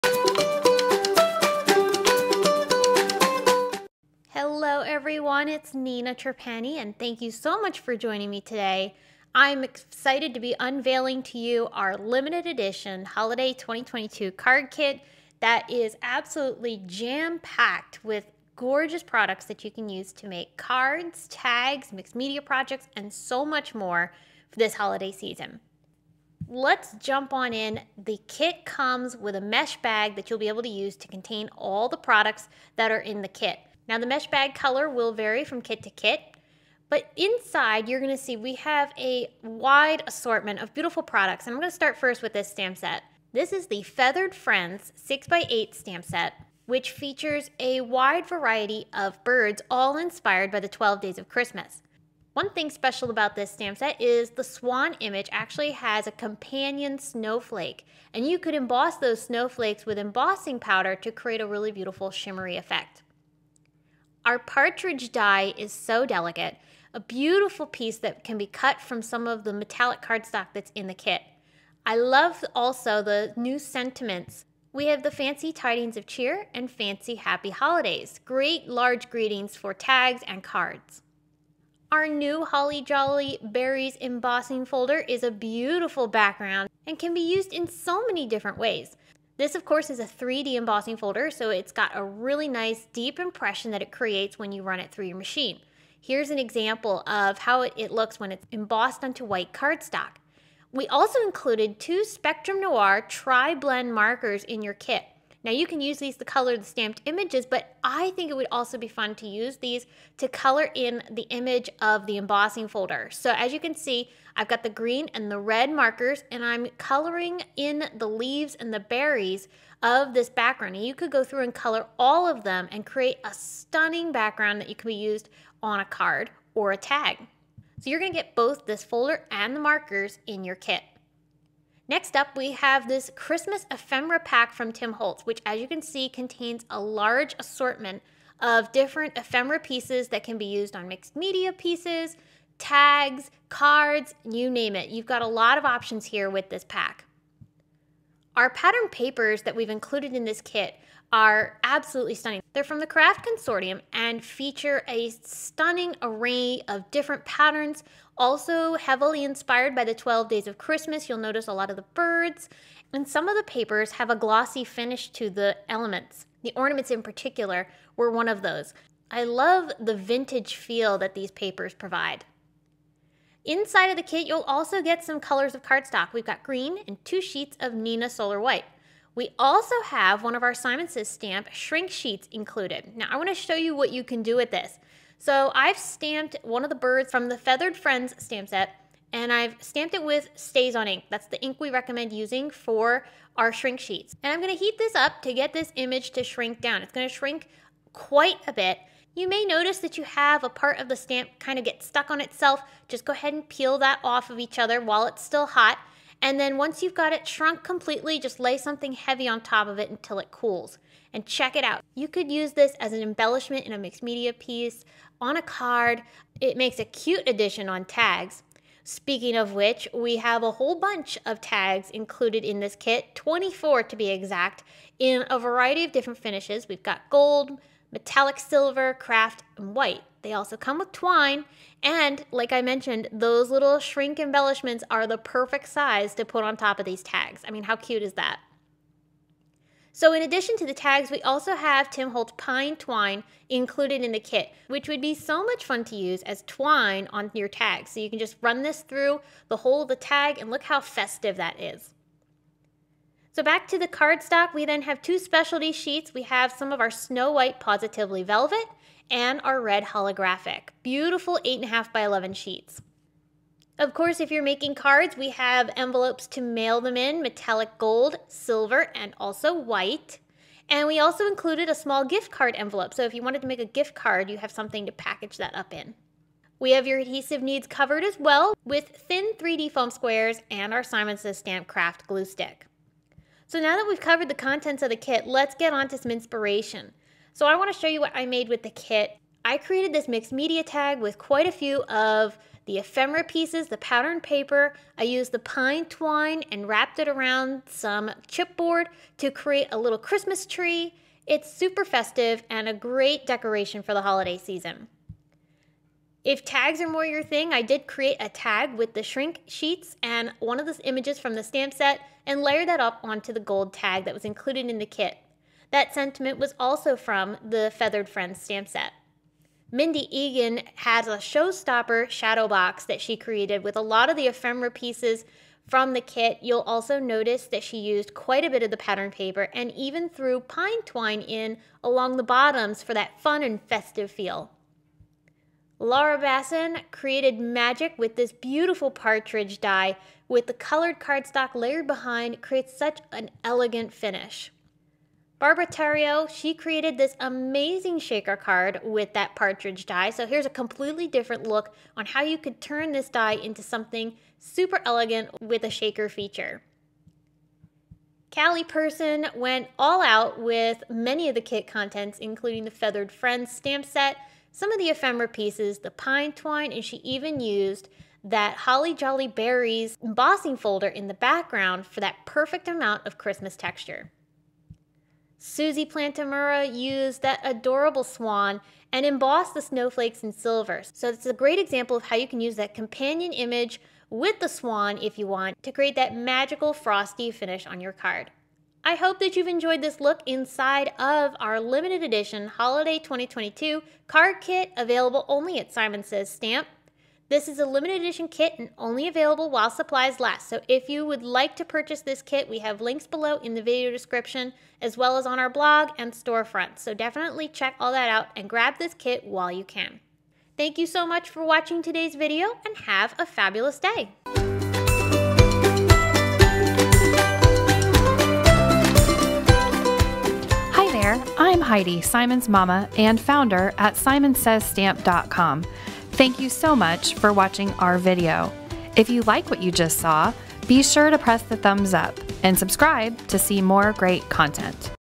DON'T MISS IT!! Limited Edition Holiday 2022 Card Kit Reveal
Walk through our Limited Edition Holiday 2022 Card Kit with Nina-Marie Trapani and see how you can use the contents to make cards, tags, mixed media projects, and more!